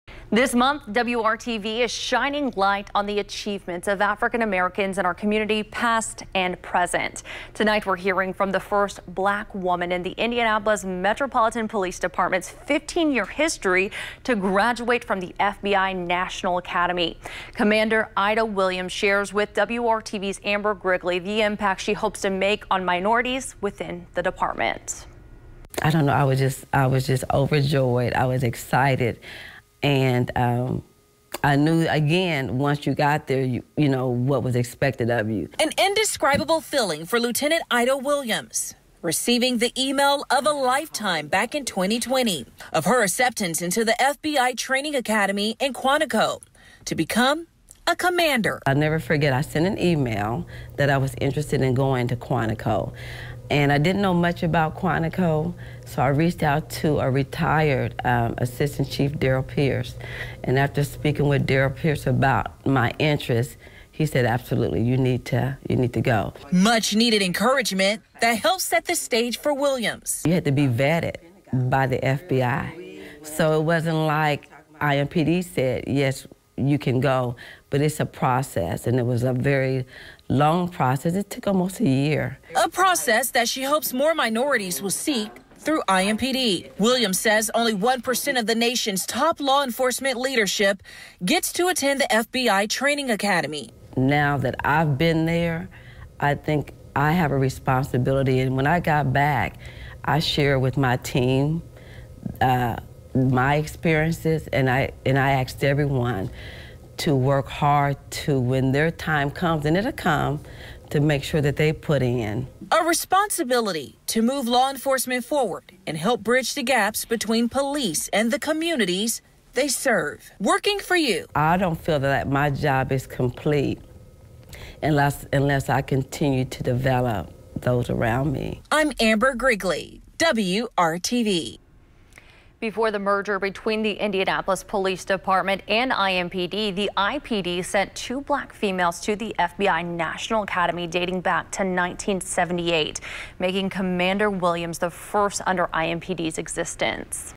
Celebrating Black History Month: Commander Ida Williams
IMPD's Cmdr. Ida Williams shares her experience being the department's first Black woman to graduate from the FBI National Academy.